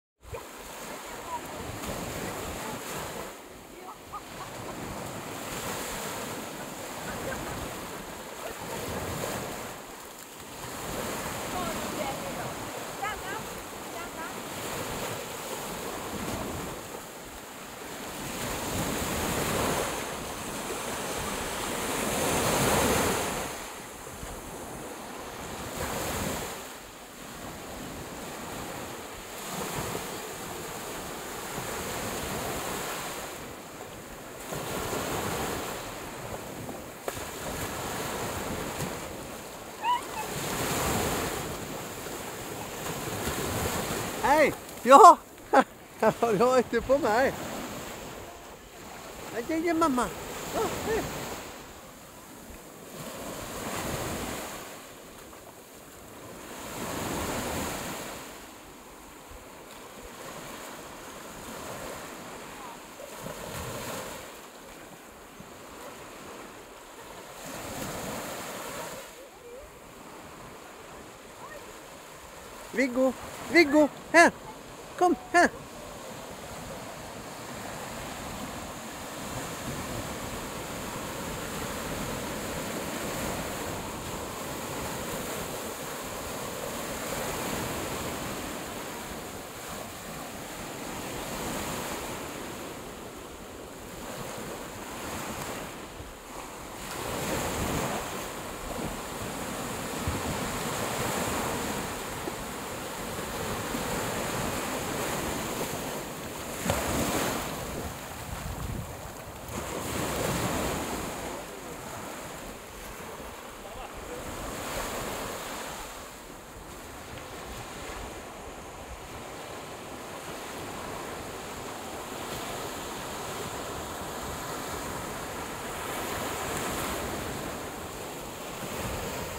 Den badande Schnauzern 🐶
Viggo har en skön inställning och nyfikenhet. 🥰